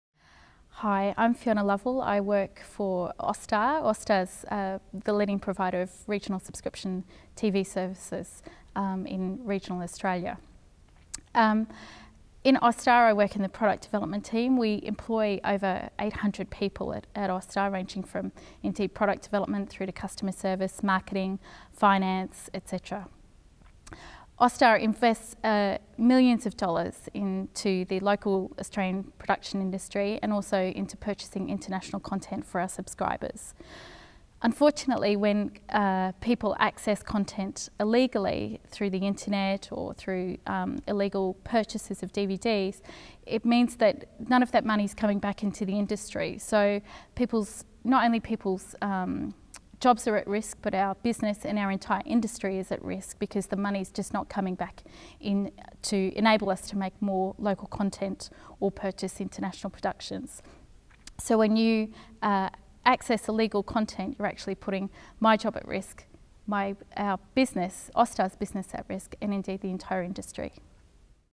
Vox Pop: Fiona Lovell, Austar, on film & TV piracy
Fiona Lovell, Product Development Manager, Austar, talks about the effects piracy has on the film & TV industry, in relation to the new Accidental Pirate campaign.

or read the blog at blog.AccidentalPirate.com.au